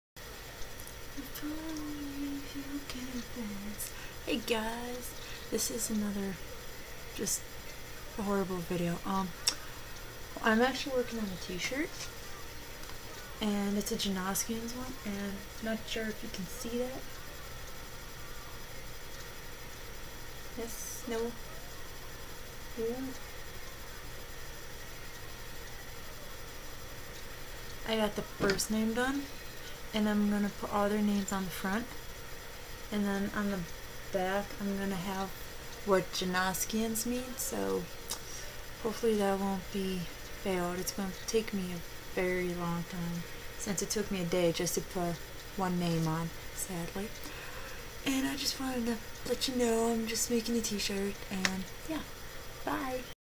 Janoskians shirt
I'm currently making a Janoskians shirt and just letting you guys know what I plan on doing.